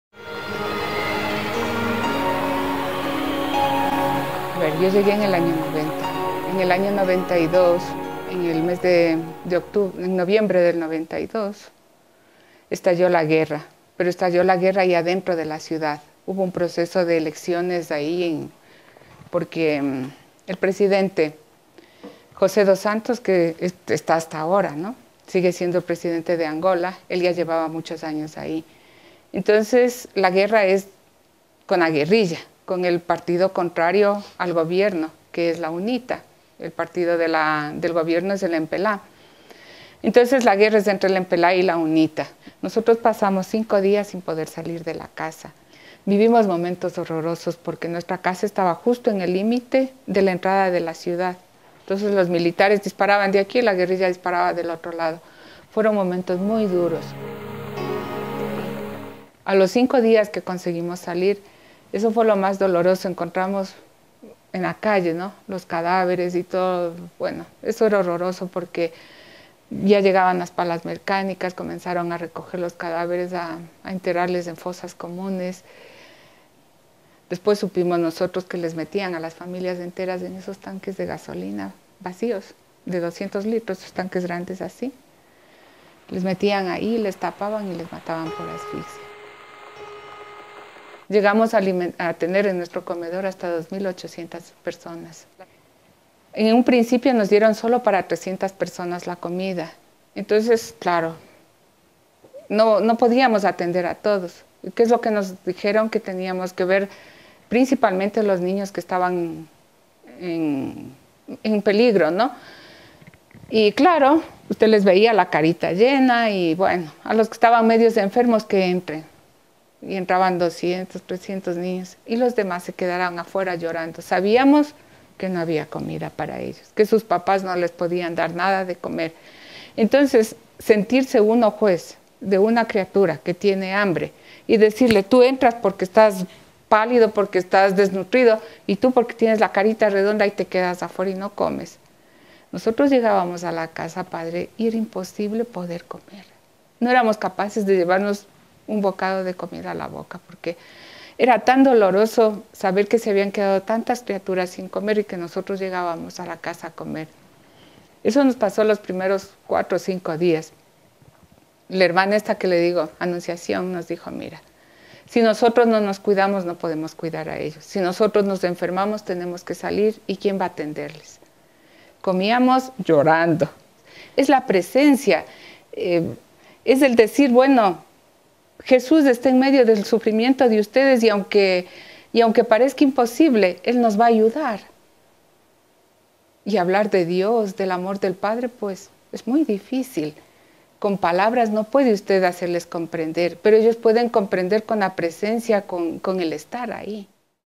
Misionera en plena guerra
En 1991, tras un largo período de guerras internas en Angola se realizaron elecciones pero el resultado de las mismas no convenció a la oposición pues denunció un fraude electoral generando nuevos enfrentamientos.
Durante esos duros momentos la Hermana mercedaria Clara Luz Gonzáles fue testigo de la violencia de ambos bandos y de el dolor de las víctimas que esta guerra produjo. Ella misma sufrió el hambre, la angustia y la incertidumbre de no saber que pasaría. En medio de estos problemas su fe la mantuvo en ese lugar pues sabìa que al menos allí podía acompañar a la gente a aliviar en la medida de sus posibilidades el dolor que les aquejaba.